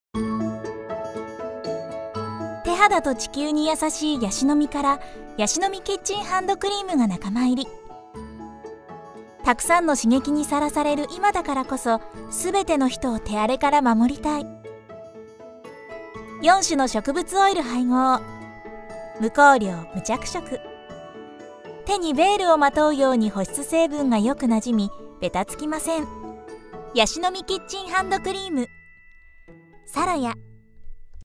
ヤシノミ キッチンハンドクリーム 手肌をまもる編
▼ヤシノミ キッチンハンドクリーム 商品ページ

手肌と地球にやさしいヤシノミ洗剤から、ヤシノミキッチンハンドクリームが仲間入り。手にヴェールをまとうように保湿成分がなじんでベタつきません。